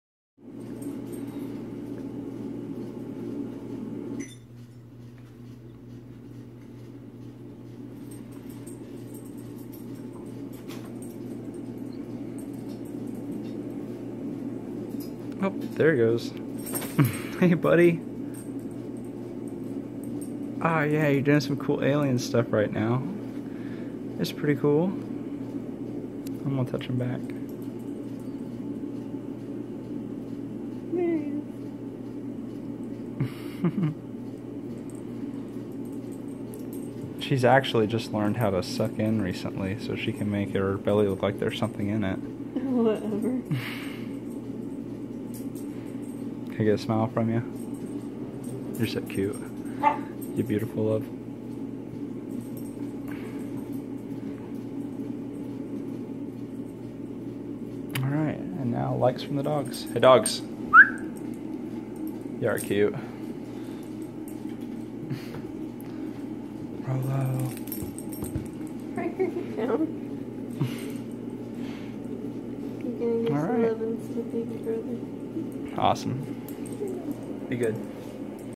BABY KICKING!
Declan kicking mikaylas side!